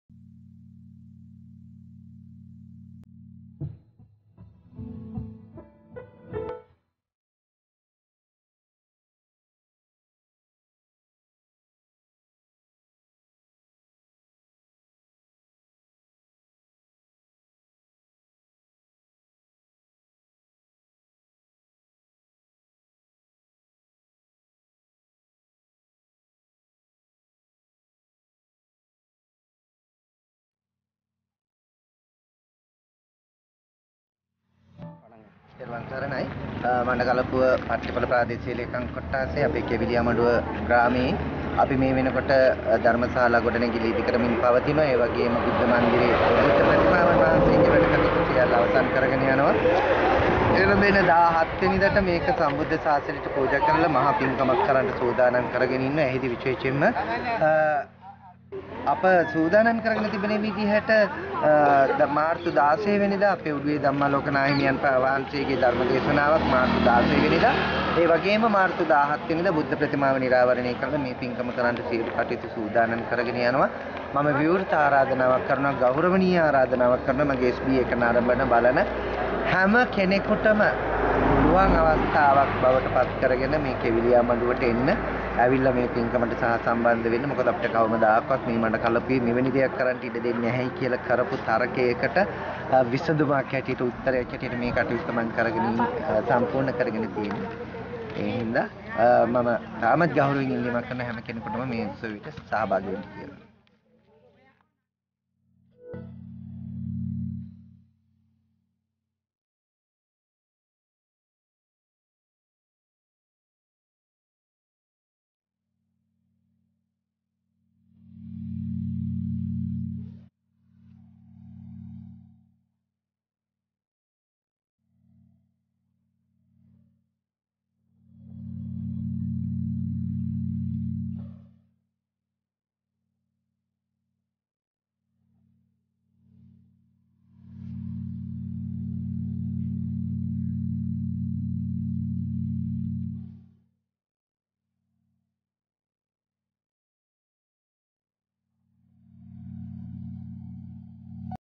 2019 03 17 වන දින පිංකම් මාලාව සදහා බැතිබර ඇරයුමයි..
බැතිබර ඇරයුමයි...
මඩකලපුව කෙවිලියාමඩුව “පන්සල්ගල ශ්‍රි අභිනවාරාම විහාරස්ථානයේ“ ඉදිකරන ලද බුද්ධ ප්‍රතිමා වහන්සේ පාදාභිවන්දනය උදෙසා නිරාවරණය කිරීම, ධම්මකාමී සුපින්වතුන් හට ධර්ම රසය විදිනු පිණිස ඉදිකරන ලද ධර්ම ශාලා මන්දිරයයේ දොරටු විවර කිරීම සහ ගොවි රජදරුවන්ගේ කෙත්වතු සරු සාර කිරීම සදහා ඉදි කළ වැවේහි නිල් දියවර මුදාහැරීමේ මංගල්‍යය...

2019 මැදින් මස 17 වන රිවි දිනට යොදාගෙන ඇති බව ශාසනාලයෙන් යුතුව දැනුම් දෙමි...
එදින වැඩසටහන් විස්තරය

ප.ව. 02:30 ට බුද්ධ ප්‍රතිමා වහන්සේ නිරාවරණය කිරීම
ප.ව. 03:00 ට වැවෙහි මංගල දියවර මුදාහැරිම
ප.ව. 03:30 ට ධර්ම ශාලා මන්දිරය විවෘත කිරීම
ප.ව. 03:45 ට උත්සව සභාව ආරාම්භය
ප.ව. 06:30 ට අභිනව බුද්ධ ප්‍රතිමා වහන්සේට මංගල ගිලන්පස පුජාව
පැවැත්විම
ප. ව. 07;00 ට අභිනව ධර්ම ශාලා මංදිරය් පැවැත්වෙන (ධර්ම දෙශනය
න
රත්‍රී 09 :30 ට සර්ව රත්‍රීක පරීත්‍රාණ ධර්ම දේශනය හා පසුදා උදැසන සියක් නමක් උදෙසා පවත්වනු ලබන සංඝගත දක්ෂිණාව.

මෙම උත්තරිතර පිංකම සදහා සහාභාගී වන මෙන් ඔබ වහන්සේට/ඔබතුමන්ට/ඔබ තුමියට ශාසනාලයෙන් යුතුව ආරාධනා කරමි..

තෙරුවන් සරණයි ..!
විමසිම් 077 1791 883 / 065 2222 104